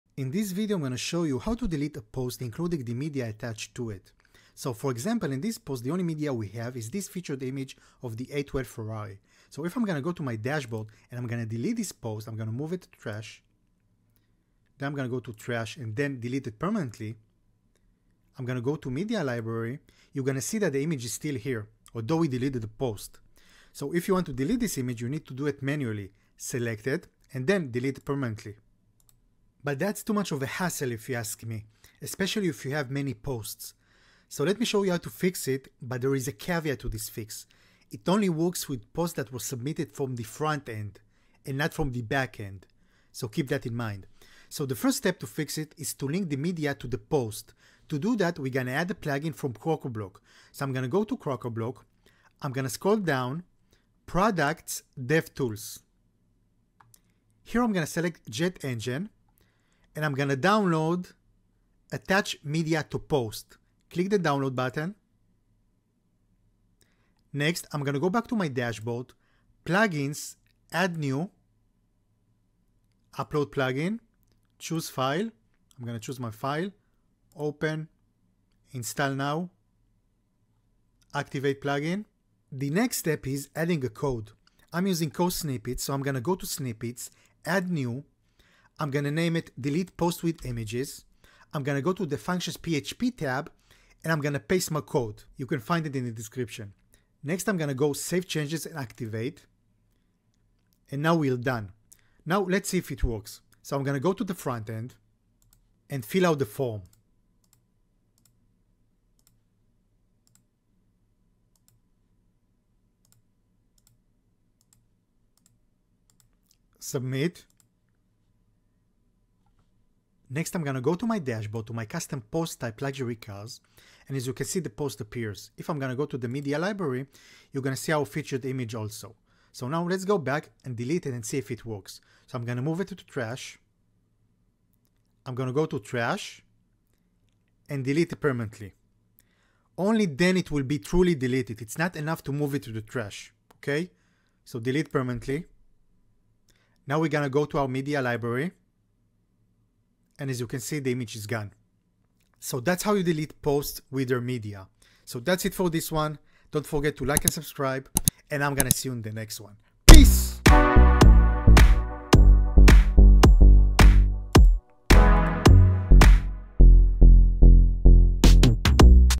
How To Delete A Post Including The Media Attached
in this video im going to show you hhoe to delete posts with thier media by ading a crocoblock plugin and a code